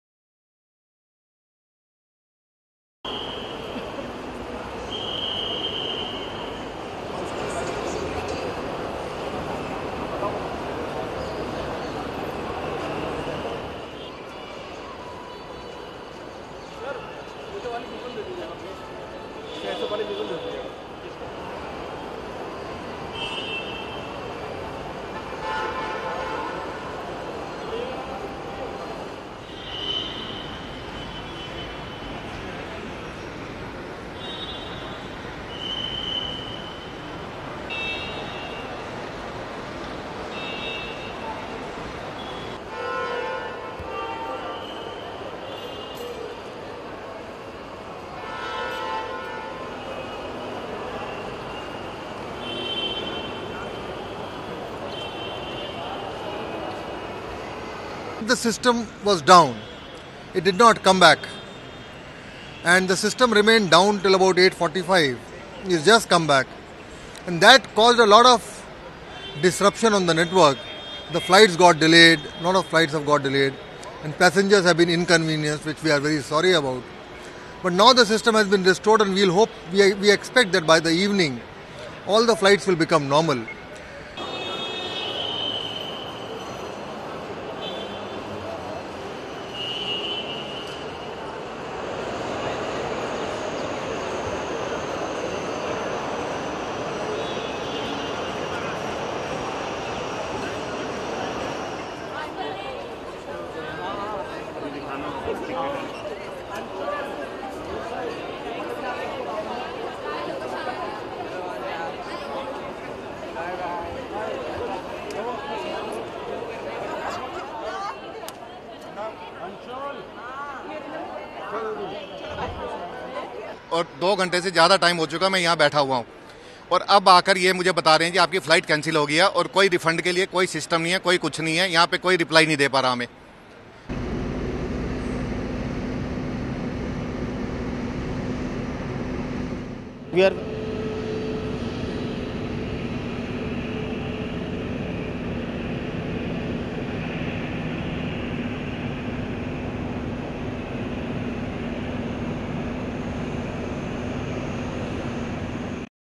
27 Apr, 2019 - Air India server systems restored after affecting flights worldwide
27 Apr, 2019 (ANI) - Air India server systems restored after affecting flights worldwide